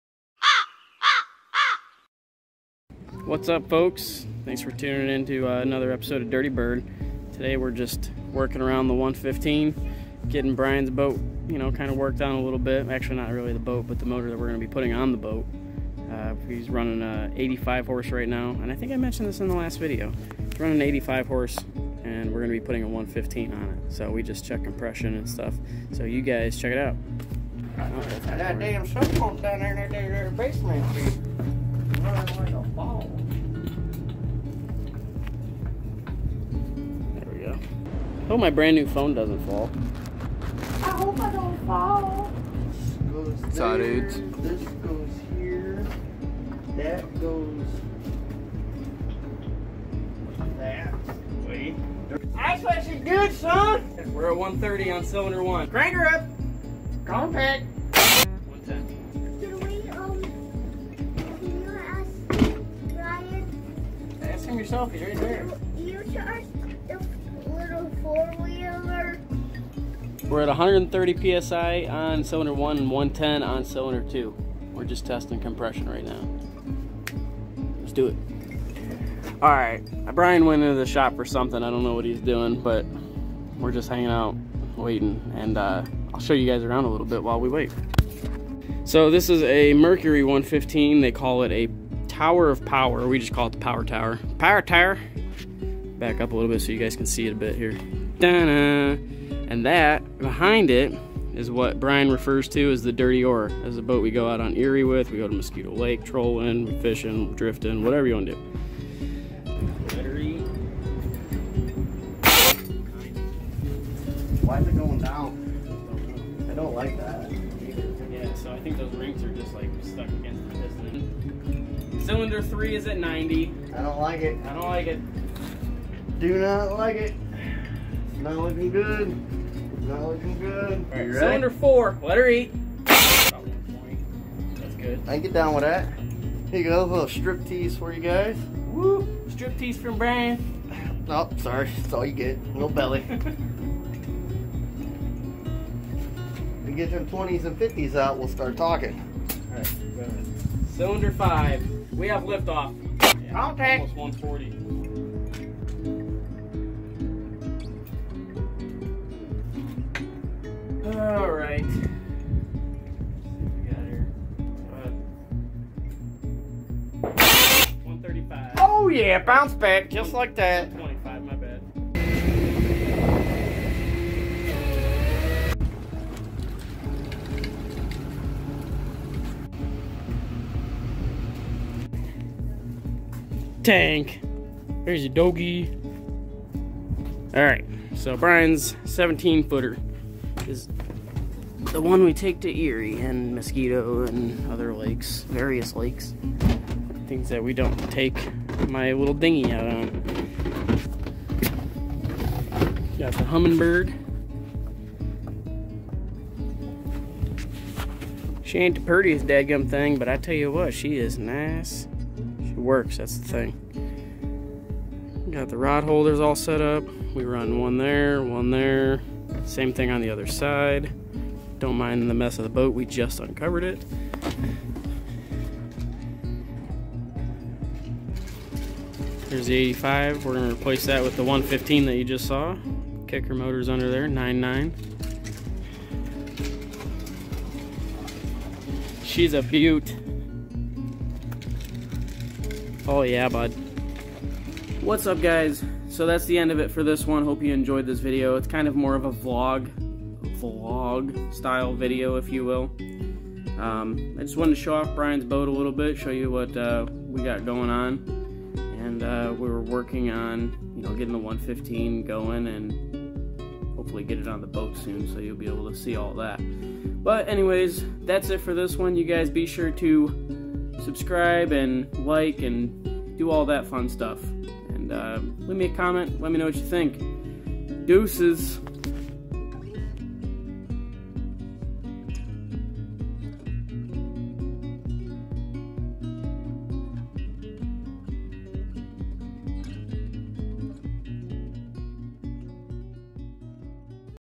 115 POWER TOWER mercury
CHECK THIS OUT. Were going to be engine swapping Bryans boat which is known as the "Dirty Oar". Here we are checking compression and also I show a little bit of the Dirty Oar itself right after we uncover it from its wintery slumber.